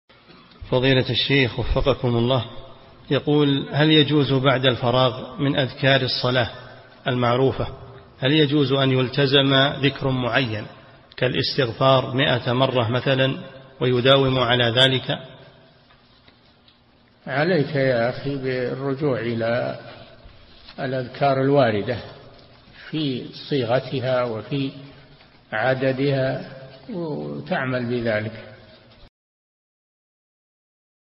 [557 -939] هل يجوز بعد الفراغ من أذكار الصلاة التزام ذكرا كالاستغفار مائة مرة، ويداوم على ذلك؟
-[557 -939] السؤال: هل يجوز بعد الفراغ من أذكار الصلاة المعروفة هل يجوز أن يلتزم ذكرا معين كالاستغفار مائة مرة مثلا، ويداوم على ذلك؟
الجواب: عليك يا أخي بالرجوع إلى الأذكار الواردة في صيغتها وفي عددها وتعمل بذلك.
للاشتراك في قناة الشيخ صالح الفوزان

للاستفسار والإقتراحات والدعم
رئيس الفريق
لمشروع كبار العلماء